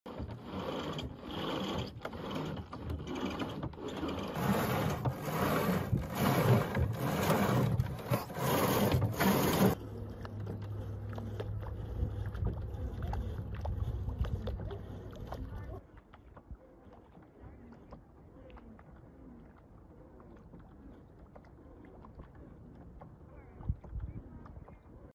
Man builds pedal-powered skiff
This Monk plywood skiff is wonderfully complex and is propelled by innovation.